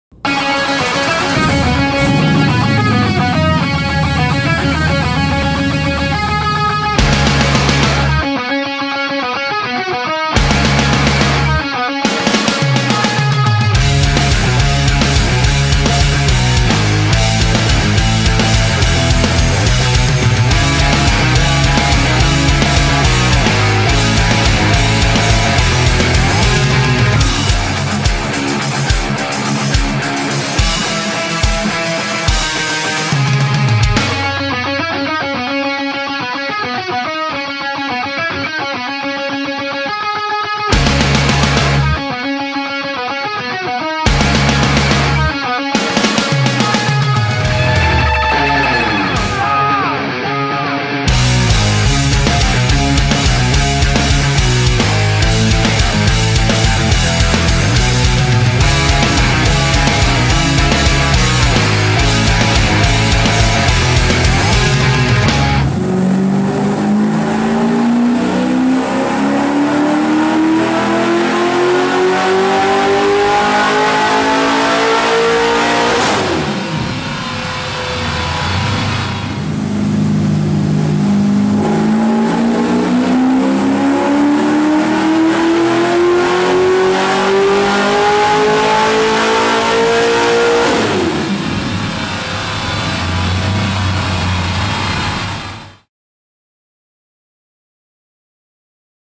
Tuning the Driftvette - Lingenfelter Performance and Engineering
Formula Drift driver Dirk Stratton getting his Lingenfelter powered driftvette tuned out here for his upcoming event in St. Louis this weekend! Be sure to check it out!
Fortune Auto
Achilles Tires USA
Grip Royal
McLeod Clutches
ENKEI WHEELS
Oracle Lighting
Wilwood Disc Brakes
Stratton Chevrolet